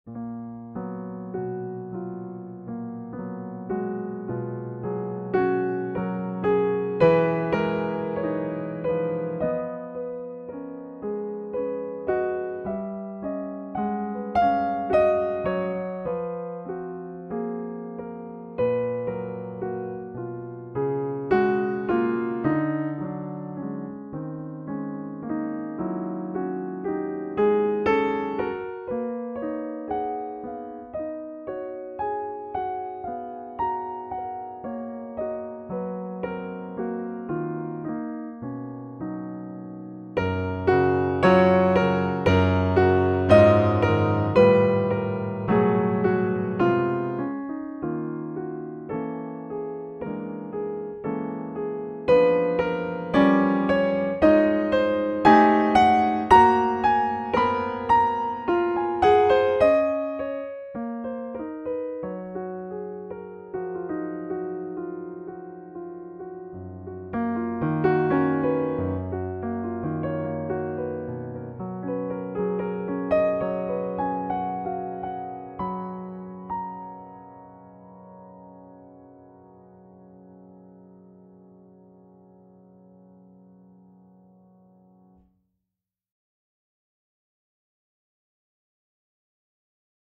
impro395
Improvised pianoforte music. Classical base with a touch of Jazz and contemporary piano. No editing. Played on a Roland master keyboard with sounds courtesy of Modart Stage Software, Steinway model D preset.
 Acoustic tracks played on Bluthner grand and Bechstein upright.
For more tracks in high quality wav format please visit my Soundcloud profile